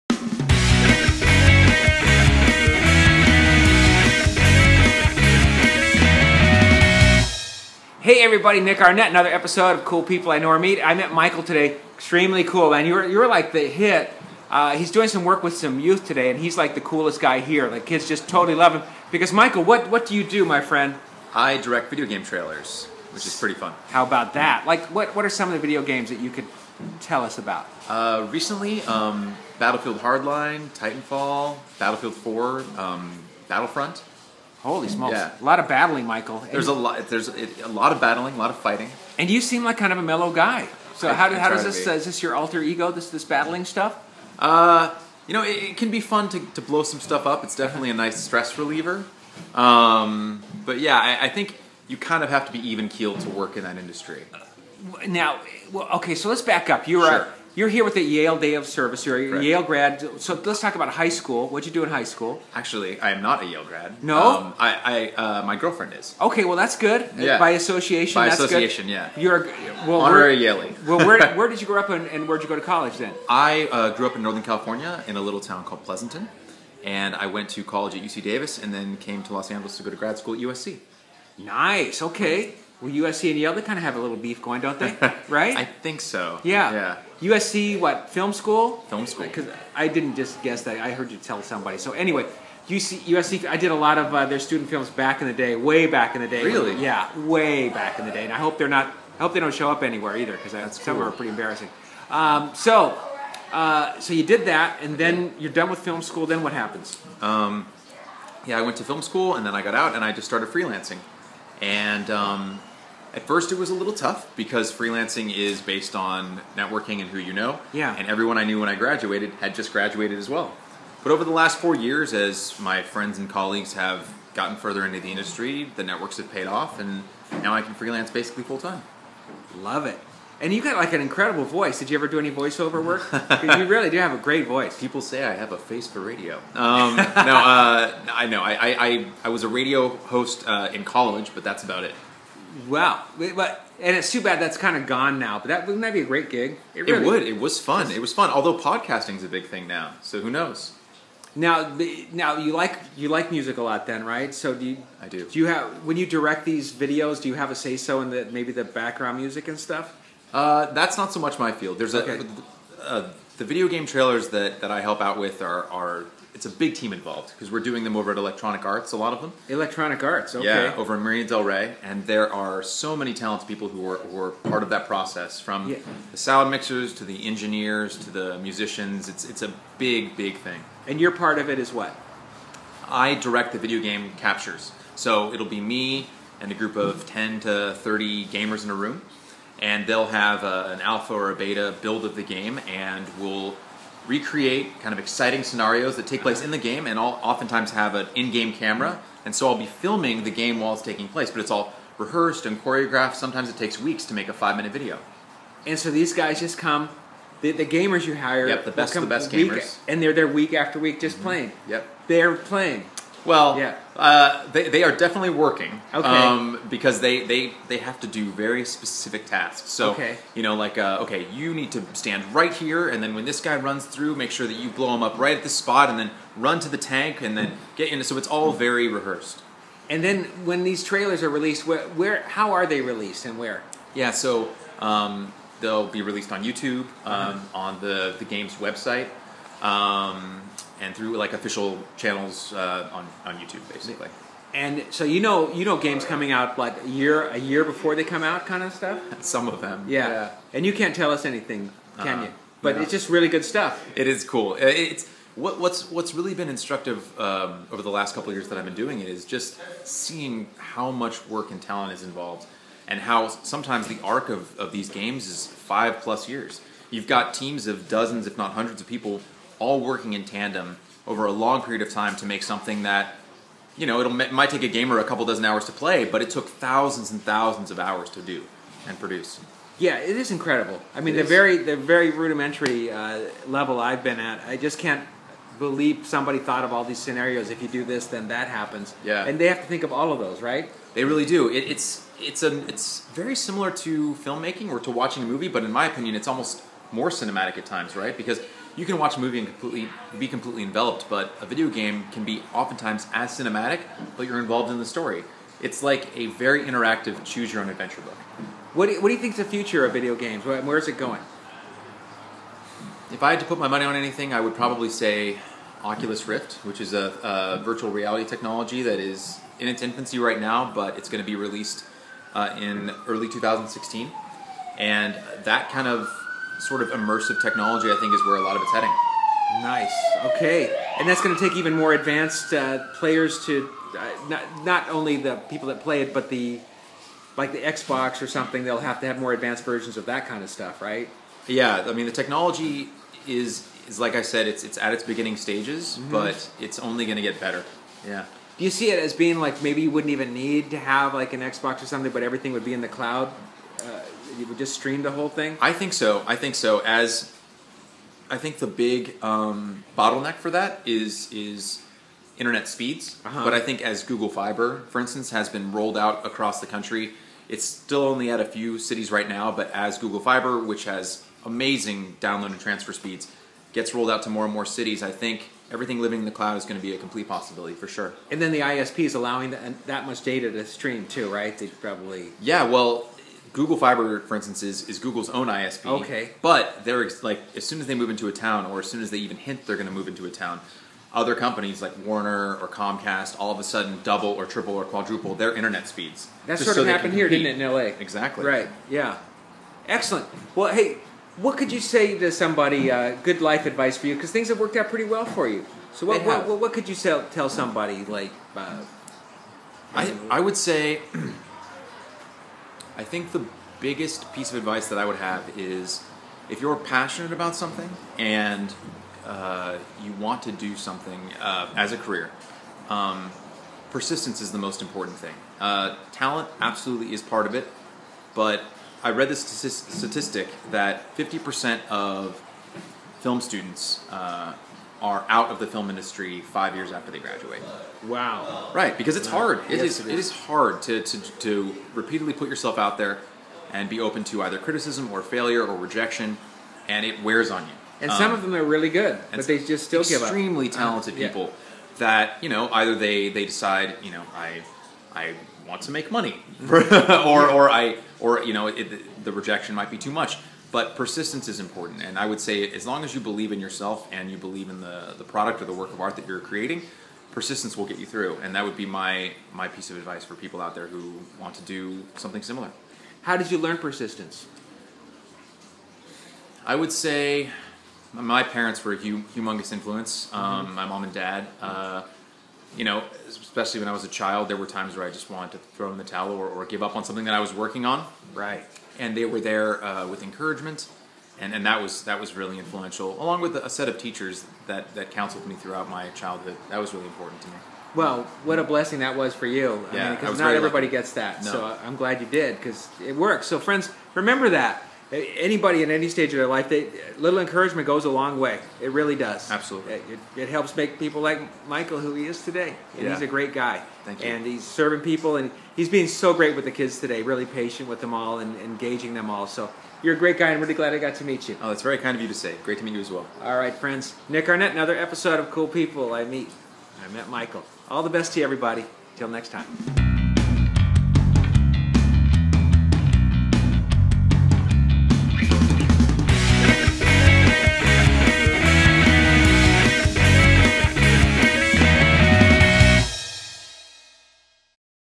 Cool People I Know or Meet, with Nick Arnette-Episode 30-Michael Callahan
Meet video game trailer director Michael Callahan. Find out how video game trailers are made and the importance of persistence.